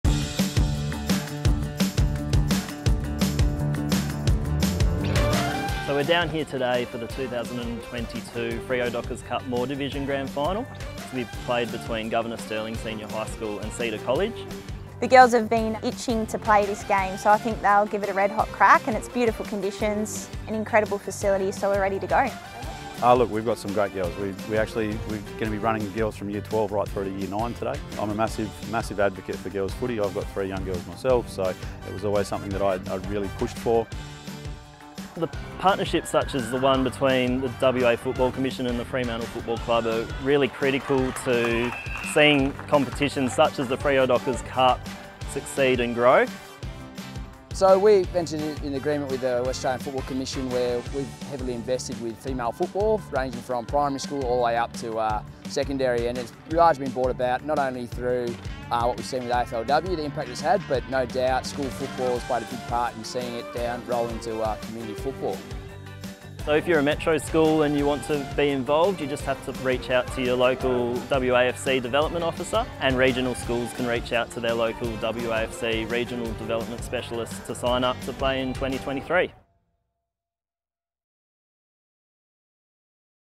Freo Dockers Cup 2022
The Freo Dockers Cup is the premier schoolgirls competition for Year 10-12 students across WA and is coordinated by the WAFC, the Schoolgirls Advisory Committee, and supported by the Fremantle Football Club.